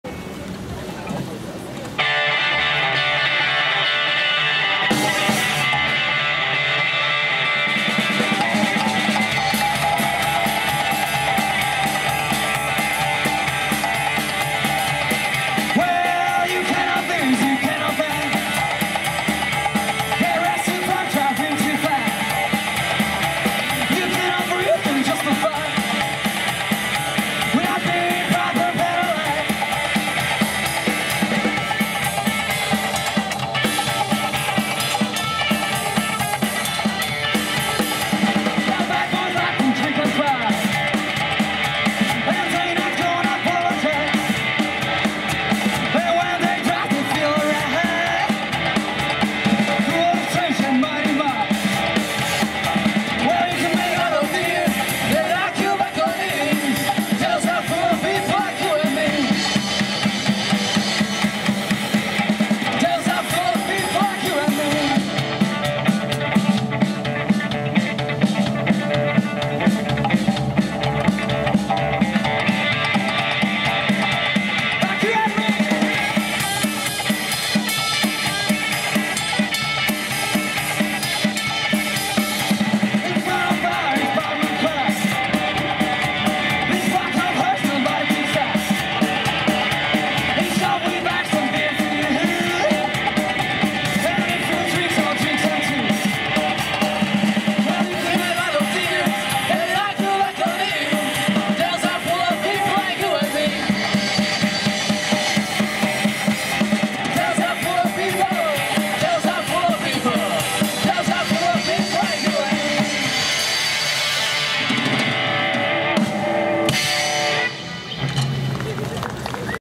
Vaudeville 2011 (reupload)
Jubilejný, už 10. ročník cyklistického podujatia pod názvom Cyklojazda histórie sa uskutočnil 2.9.2011.  Na zvolenskom námestí vítali pelotón, ktorý tam dorazil okolo 16:20 hod. netrpezliví nadšenci.

Pred samotným príchodom a po príhovoroch čestných hostí zahrala všetkým prítomným hudobná skupina Vaudeville z Martina, ktorá zabávala ľudí v Rock and rollovom štýle.